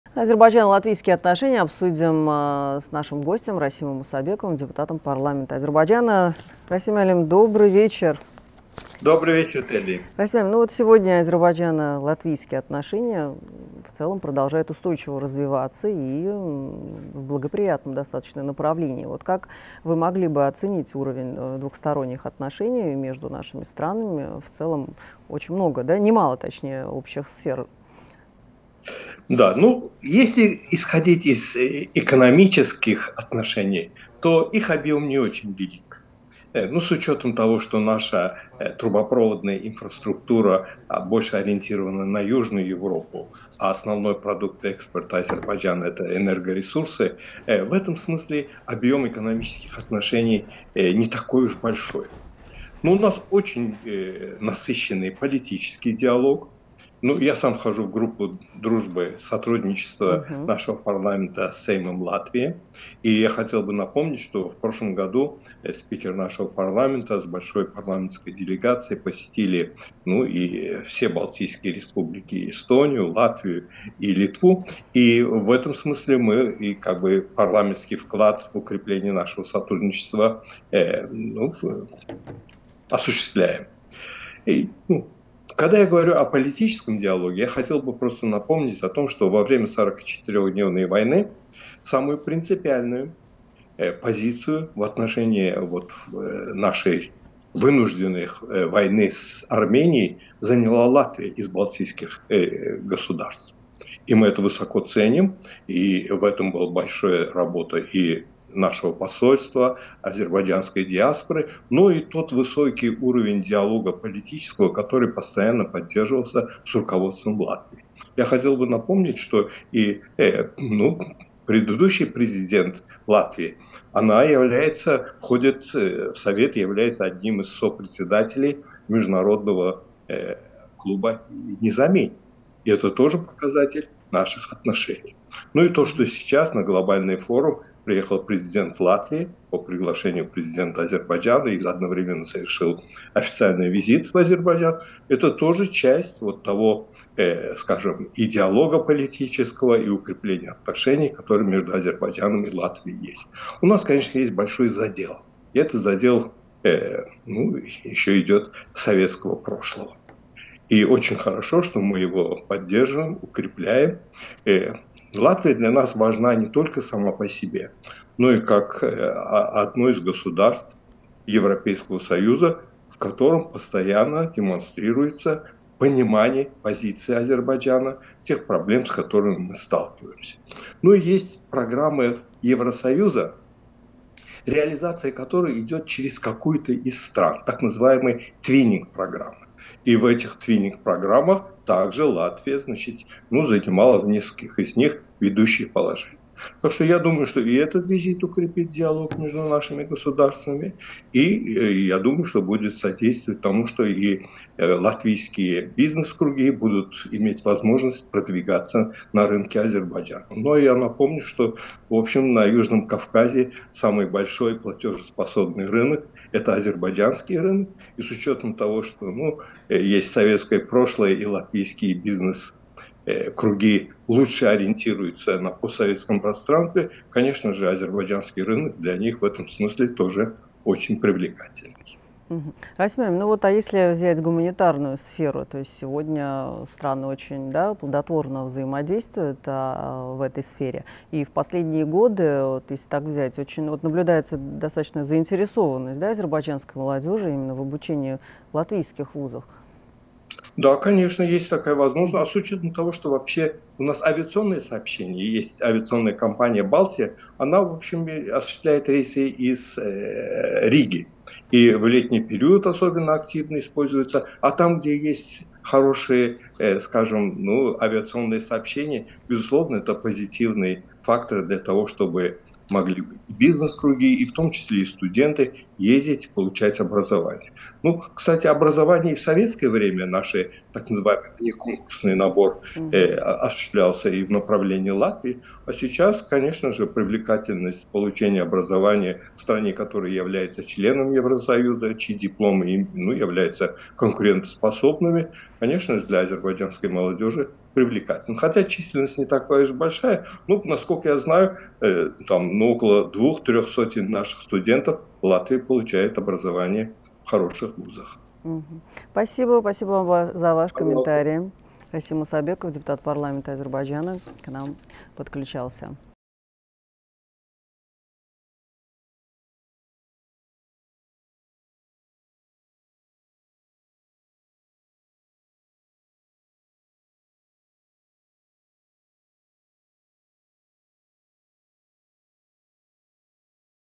Расим Мусабеков: Латвия поддержала Азербайджан в 44-дневной войне
Депутат парламента Азербайджана Расим Мусабеков об отношениях Баку и Латвии.